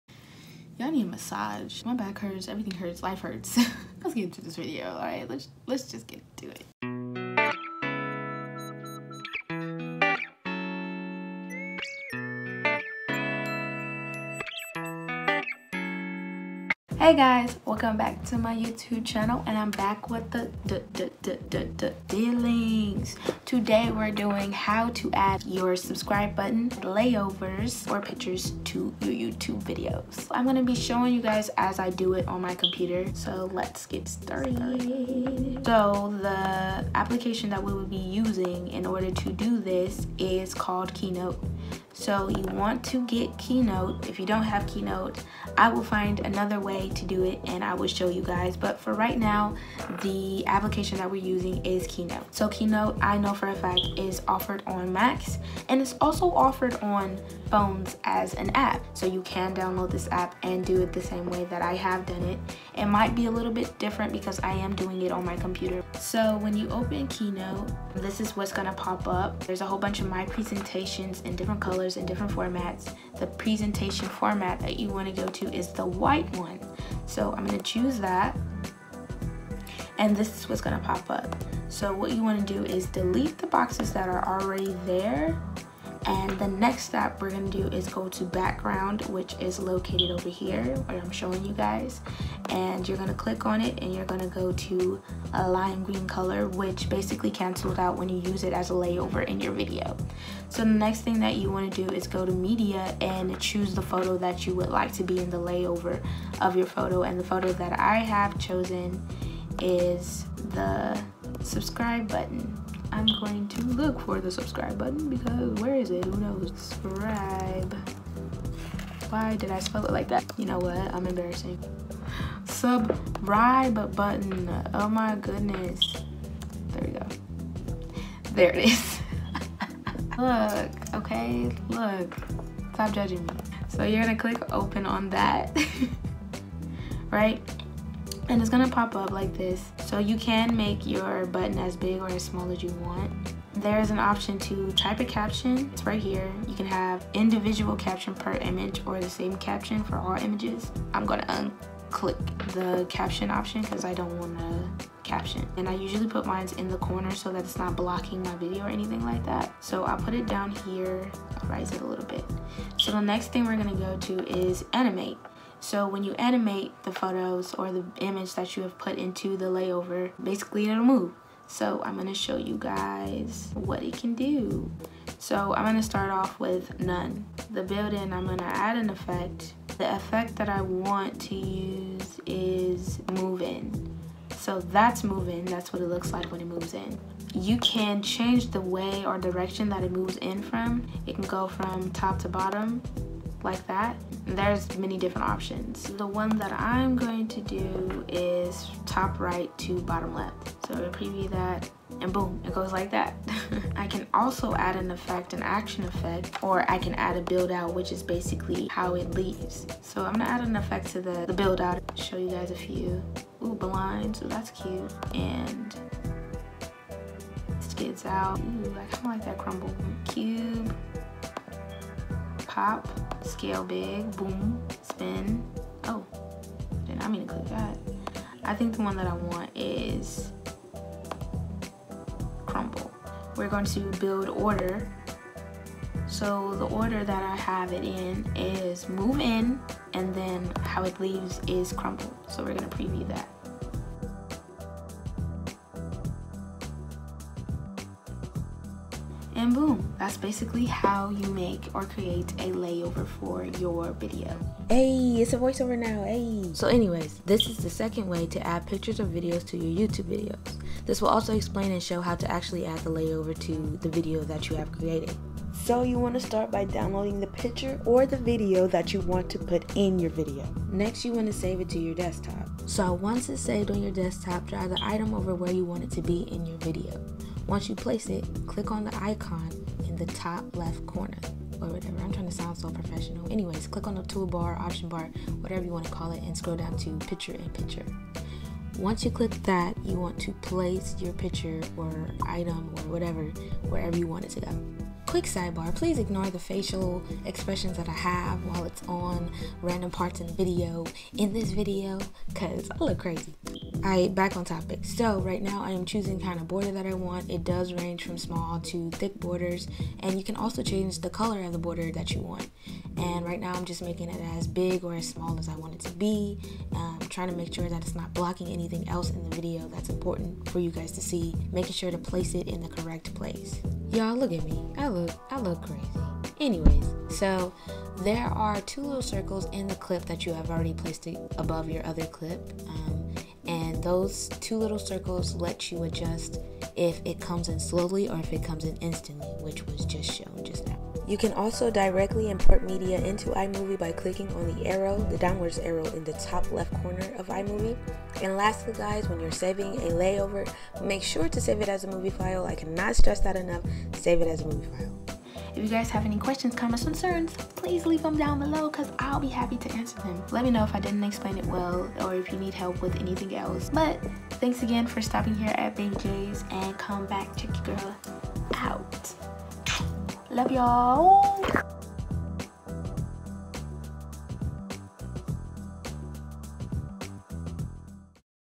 How To Add Layovers To Your Youtube Videos| Add A Subscribe Button To Your Videos |Keynote| IMovie
This video is about add layovers to youtube videos . Layovers can include many different pictures and videos! Learn how with my new video!!

Remember to save the layover as a movie file, so that you are able to add it to your videos